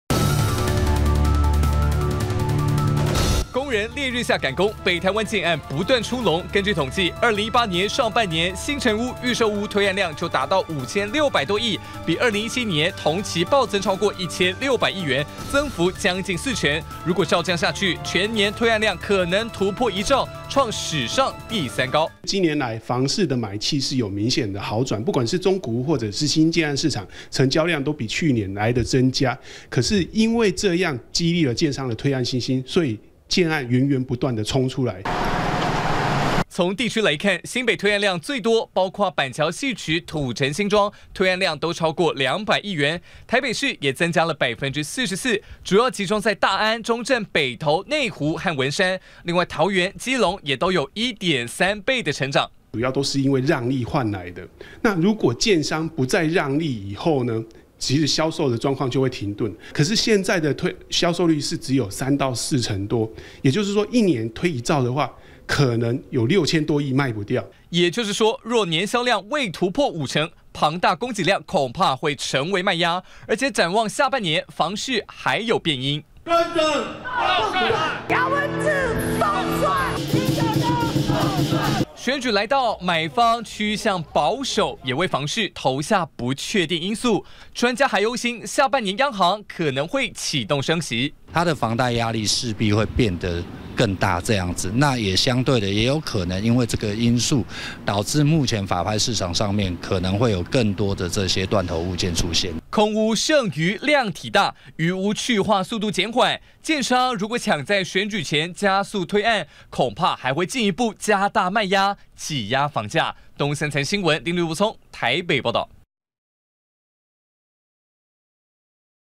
建商信心爆表！北台新屋推案將衝破1兆
工人烈日下趕工，北台灣建案不斷出籠，根據統計，2018年上半年新成屋，預售屋推案量就達到5615.56億元，比2017年同期暴增超過1600億元，增幅將近四成，照這樣下去，全年推案量可能突破1兆，創史上第三高。